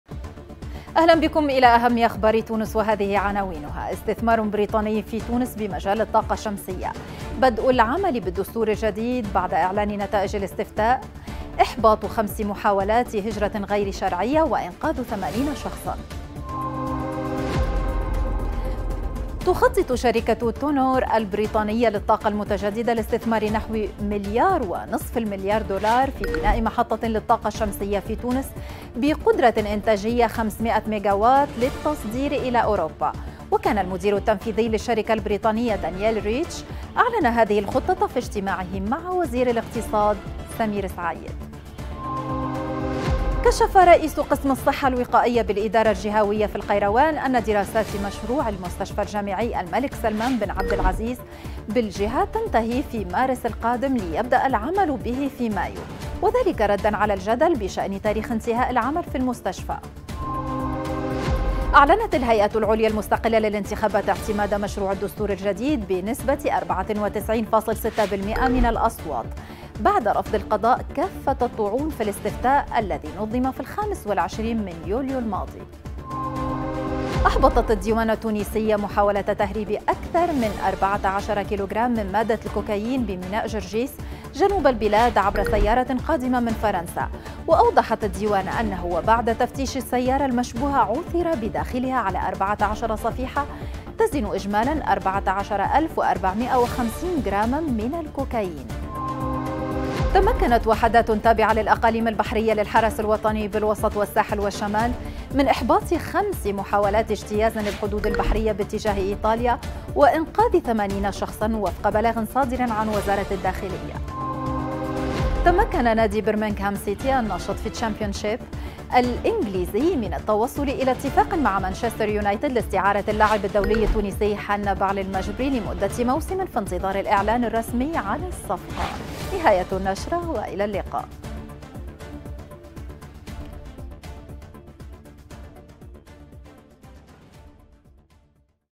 أخبار تونس 120
استثمار بريطاني في تونس بمجال الطاقة الشمسية
بدء العمل بالدستور الجديد بعد إعلان نتائج الاستفتاء النهاية
إحباط 5 عمليات هجرة غير نظامية وإنقاذ 80 مجتازا